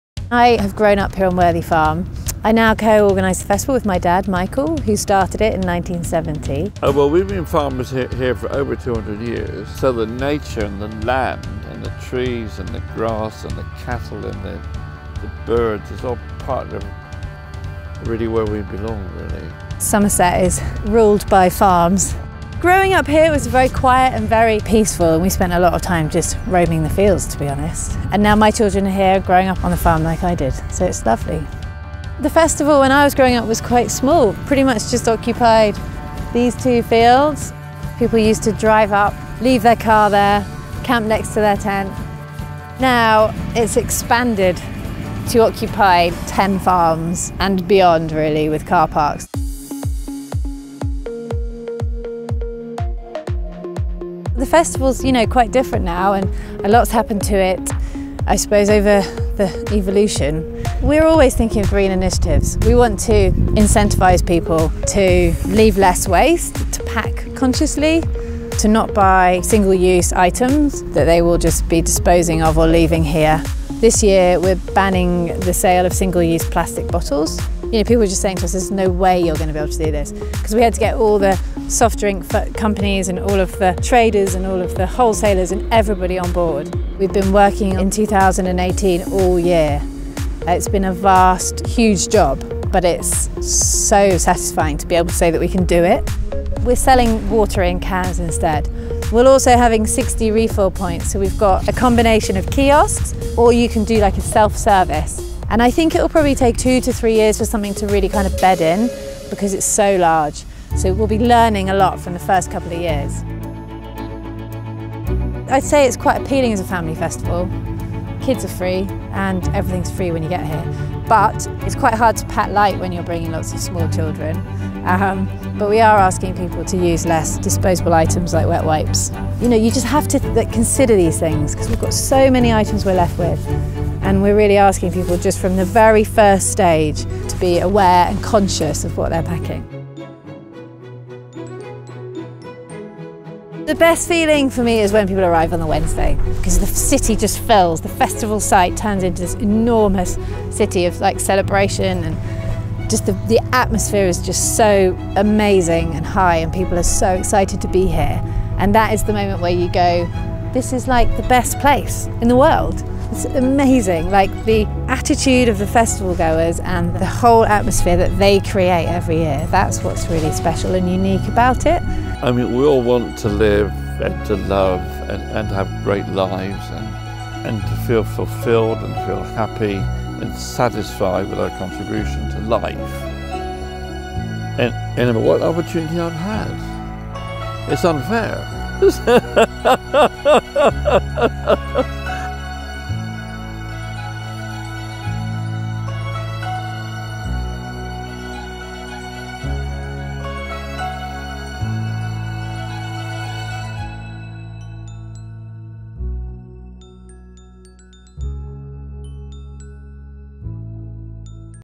How One Family's Love For The Land Became An Iconic Festival | BBC Earth At Glastonbury
Emily Eavis, co-organiser and daughter of Glastonbury founder Michael Eavis, reveals the history of the festival and its deep relationship with the nature surrounding it.